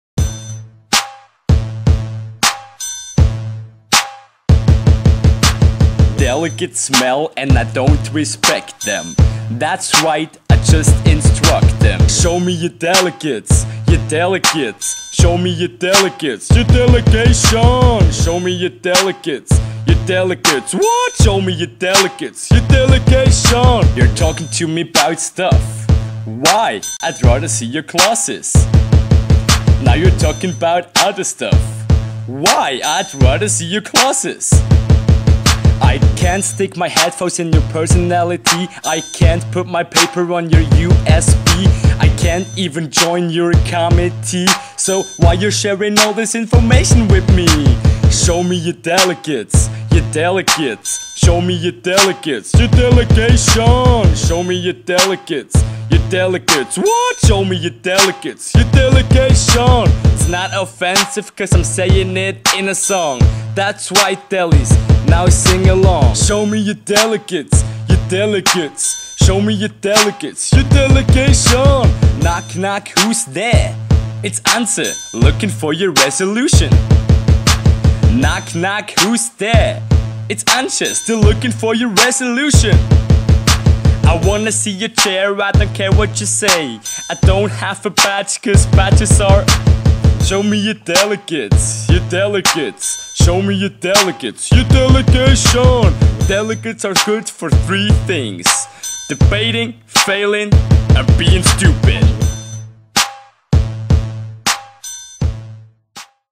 Show me your delegates
From the Leuven Lens, video magazine of the 61st International Session of the European Youth Parliament.

All respect goes to the journo's, I'm just broadcasting it for you ;)